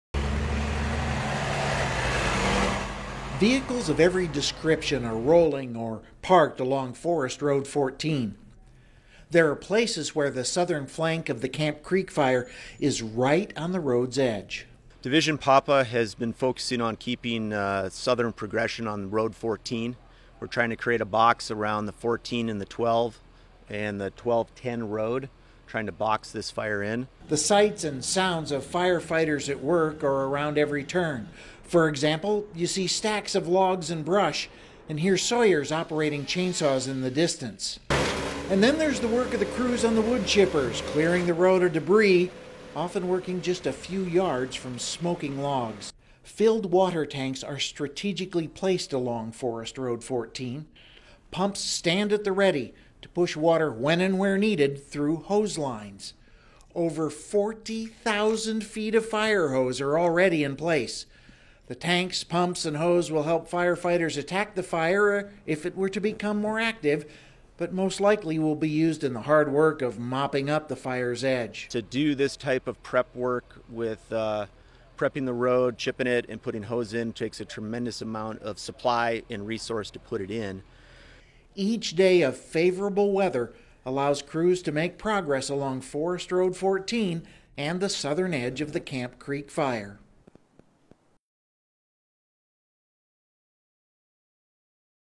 Camp Creek Fire- [Working on Road 14] 9/3/2023 Bull Run Watershed, Mt. Hood National Forest, Oregon
Camp Creek Fire @ NF-12 & NF-1210, Mt. Hood National Forest